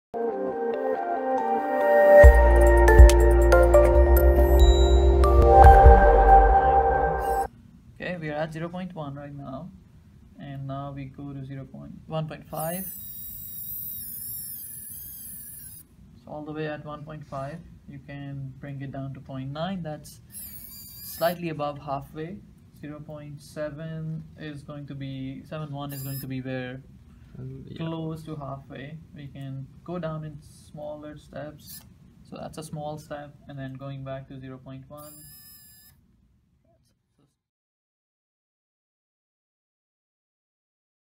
Actuonix Sponsorship: Miniature Wind Turbine
This brief clip highlights our latest sponsorship project, assembled by talented engineering students at the University Of Manitoba.

They have assembled a miniature wind turbine system that utilizes our PQ12 actuator to adjust the pitch of the blades.